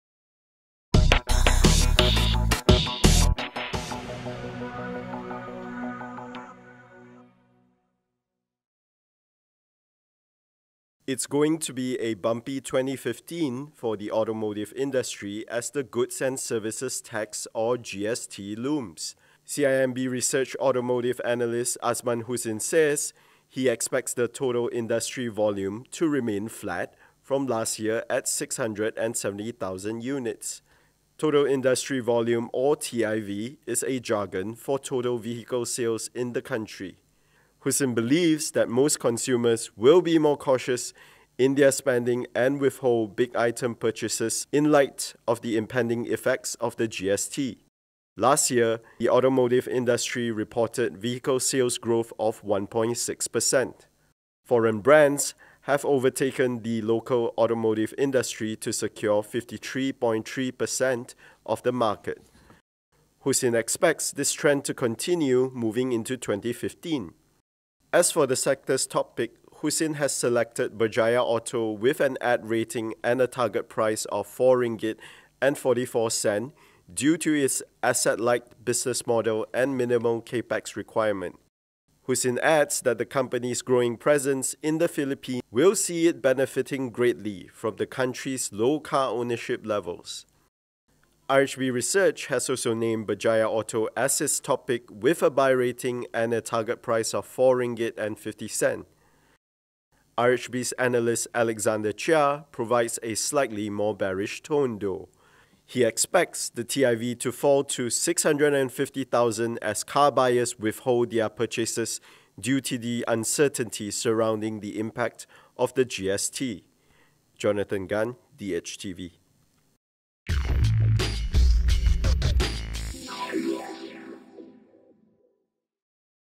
Analysts see weakening vehicles sales ahead of GST, Berjaya Auto tops sector pick
It's going to be a bumpy 2015 for the automotive industry as the Goods and Services Tax or GST looms.